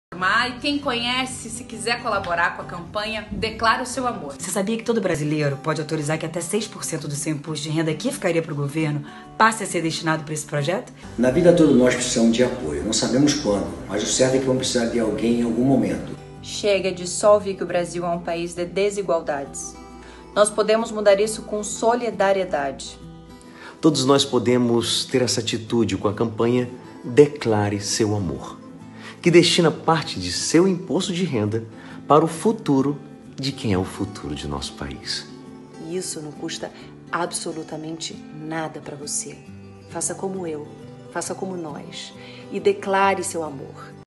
Campanha Declare o seu amor
SOBRE A CAMPANHA DECLARE SEU AMOR
 Lançada em 2018 a partir da iniciativa desse Tribunal de Justiça de Rondônia, através da Corregedoria-Geral da Justiça, a campanha objetiva levar informações a toda população sobre maneiras de contribuir com os Fundos dos Direitos da Criança e do Adolescente de todo país, com destinação de parte do Imposto de Renda de pessoas físicas e jurídicas aos fundos cadastrados.
 A aplicação dos recursos tem transparência total e fiscalização ampla de órgãos, com os parceiros, Tribunal de Contas, Ministério Público, Conselhos de Direitos, Conselhos Tutelares e o próprio contribuinte.
 Os resultados em 2018 foram altamente positivos, mas ainda há lastro para que ainda este ano, tenhamos resultados ainda melhores.